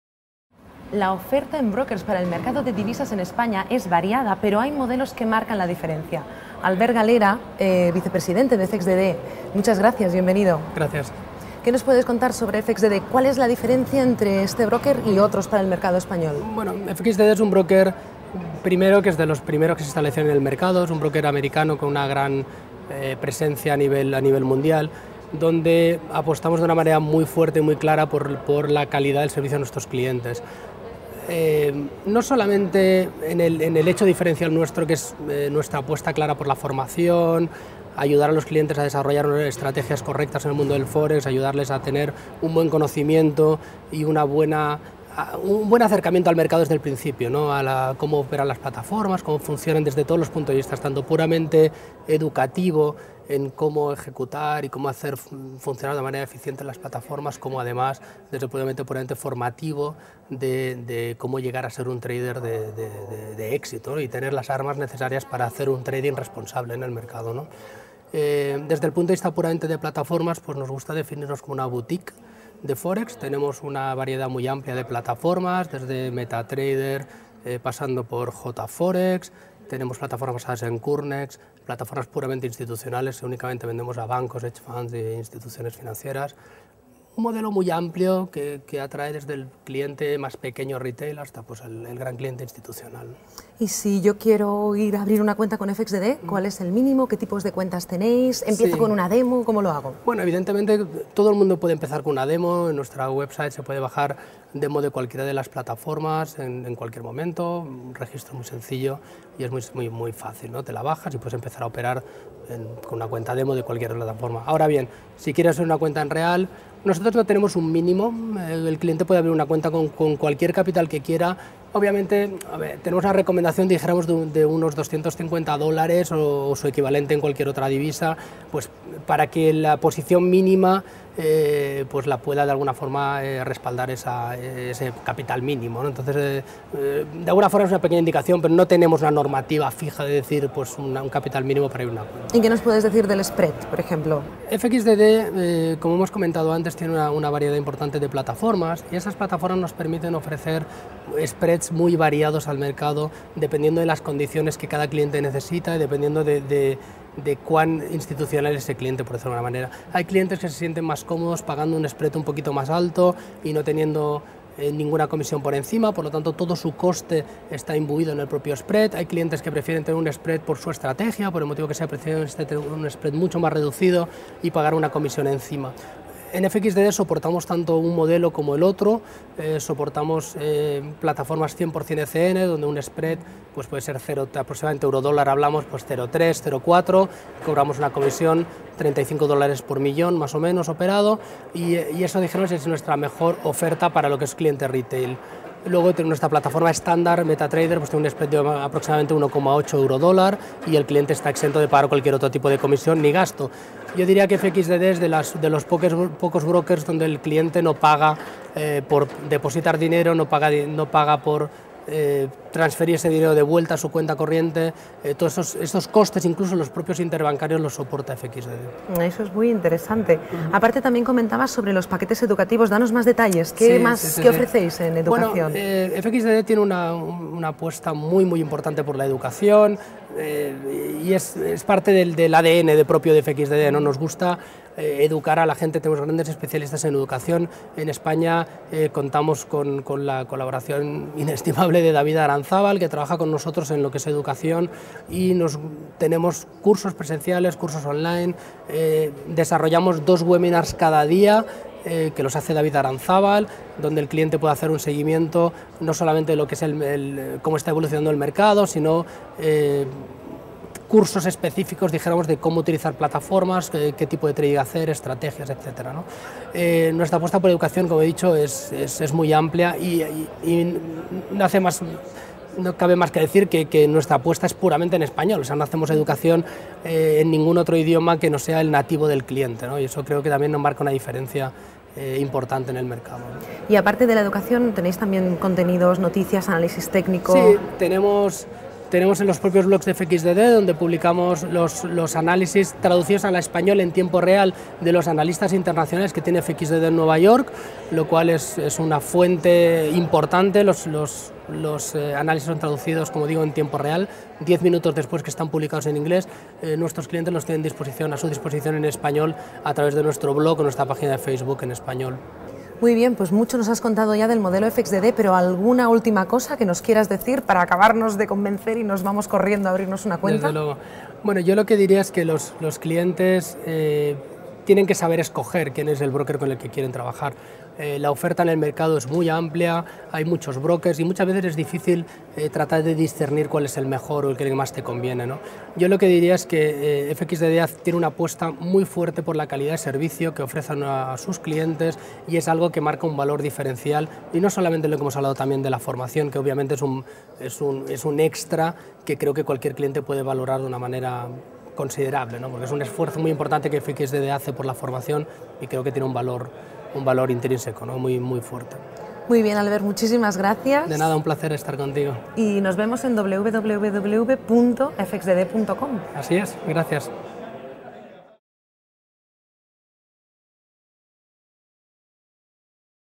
Albert Galera : Que diferencia a FXDD
Albert Galera entrevistado en el plato de TV The Forex Day.

Forex Day es el mayor evento del mundo en Castellano del Mercado Forex. Con 20 speakers, 4 de ellos de NY y Londres se celebra en Junio en Madrid.